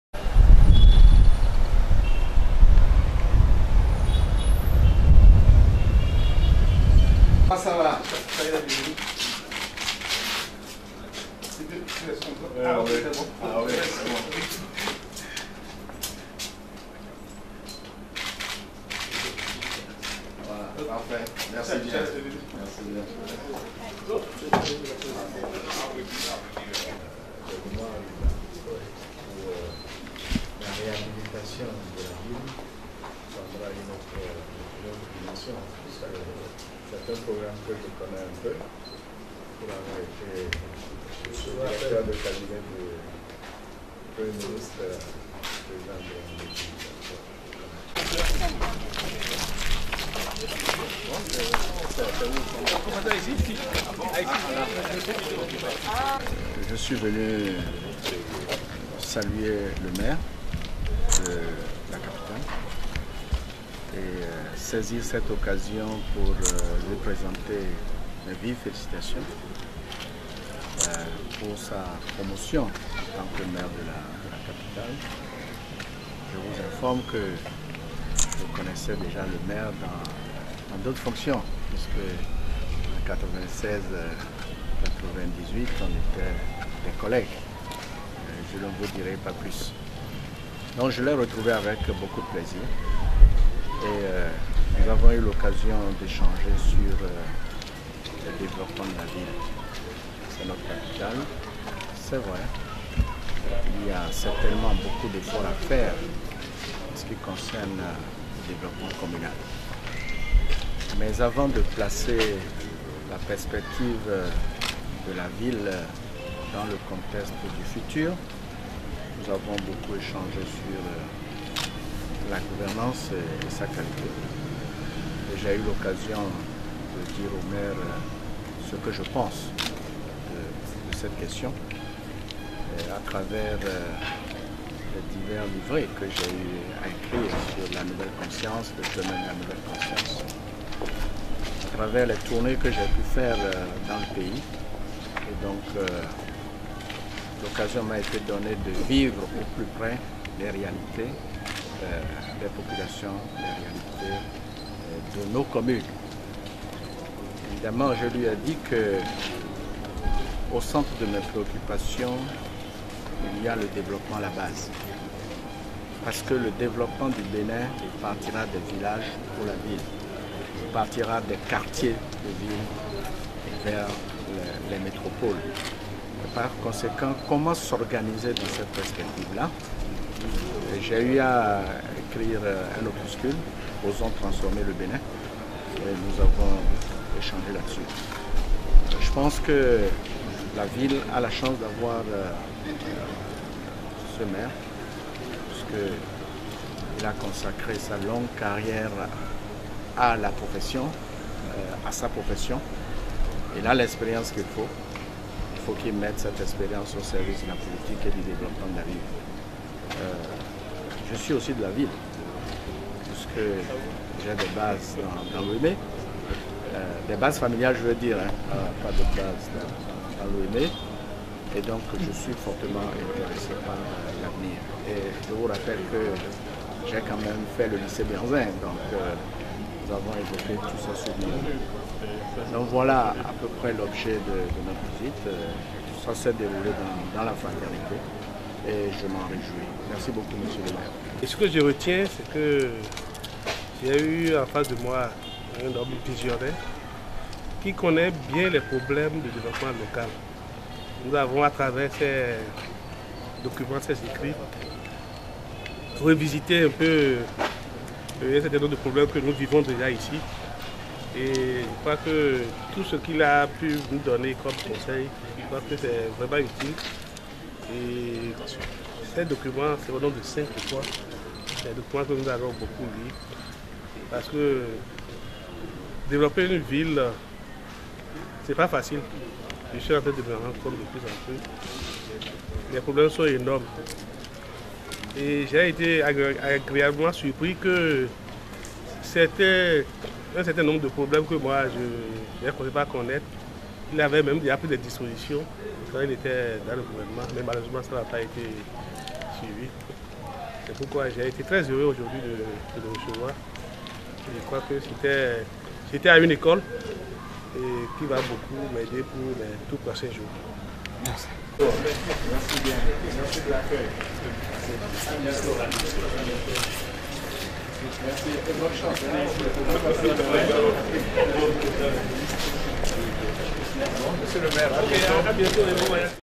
Pascal Irénée Koupaki - Visite au Maire de Porto-Novo
Visite de Pascal Irénée Koupaki au Maire de Porto-Novo